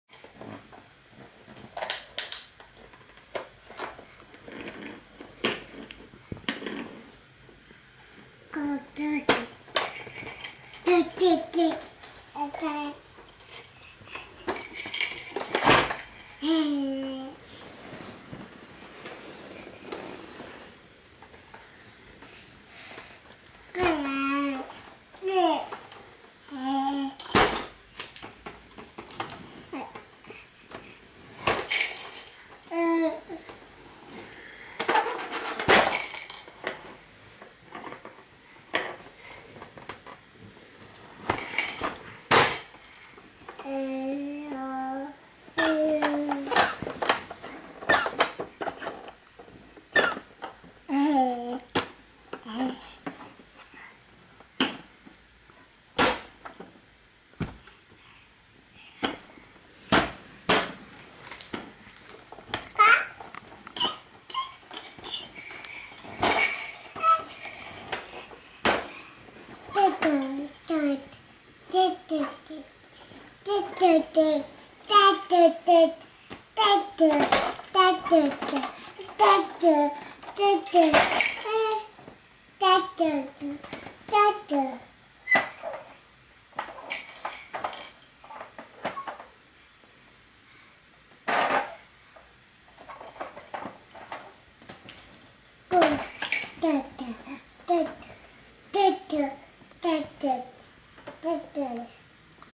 15 September (Week 58) - The activity centre that never grows old
It served Daddy, and now it's serving Little Buddy well. He never really seems to get bored of it.